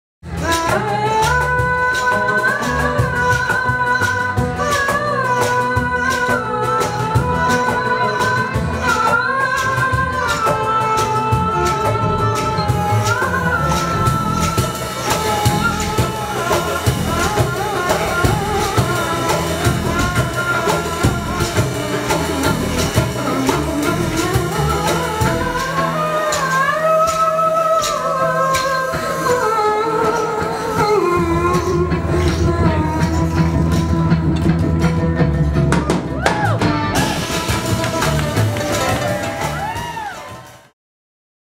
Jamale Abou Hamad | Improvisation
Live improvisation is a delight with these guys!
Musicians:
Bass: Miles Jay
Violin: Fabrice Martinez
Percussion: Luke Notary
Oud/Guitar: Gabriel Lavin